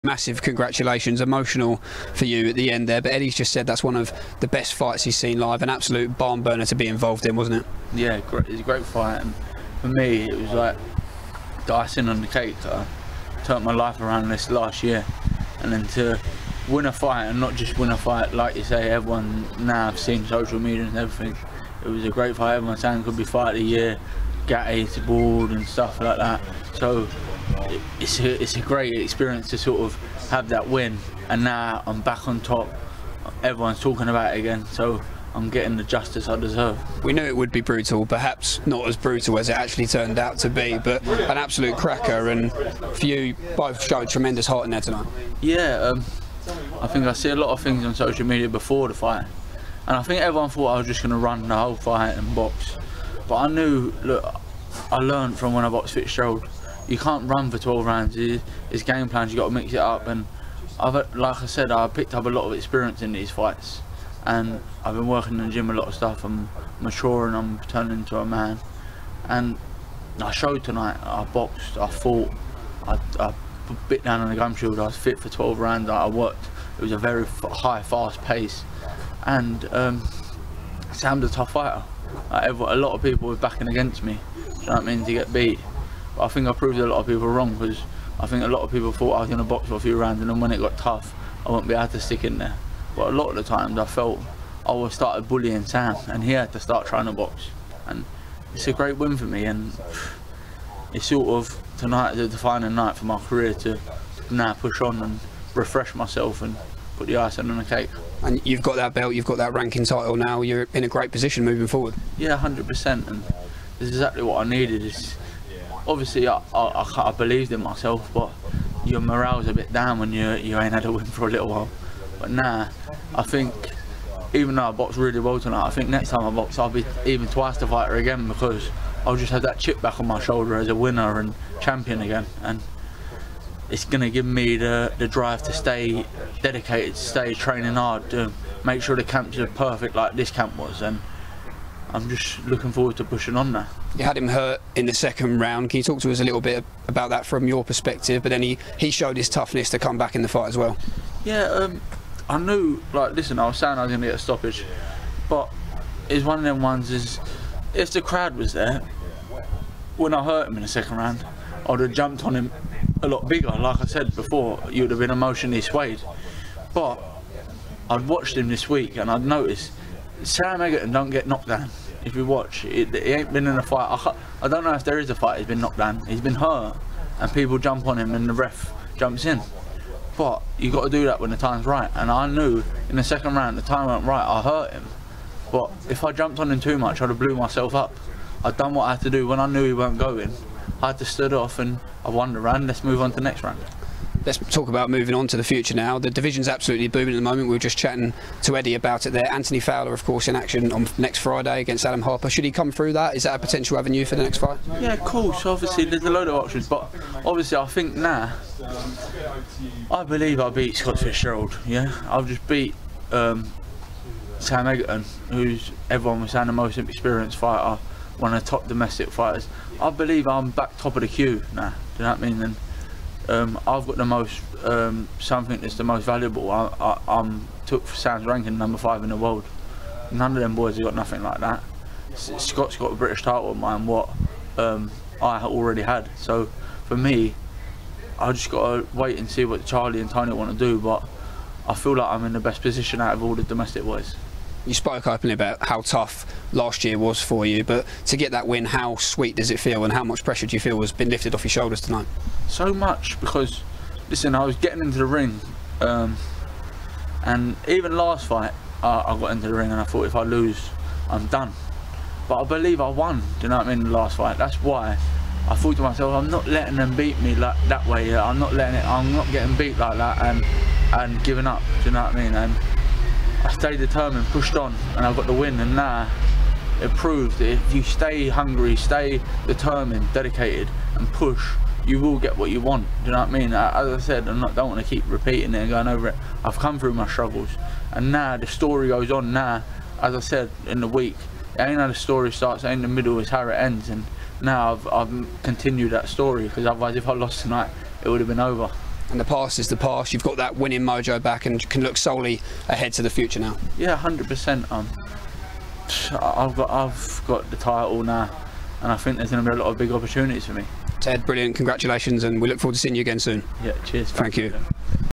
WHAT A FIGHT! Ted Cheeseman reflects on epic win vs Sam Eggington at Fight Camp
Ted Cheeseman takes home the IBF International Super-Welterweight Title after a barn burner with Sam Eggington at Fight Camp on August 1st 2020. We speak to the Big Cheese following his win...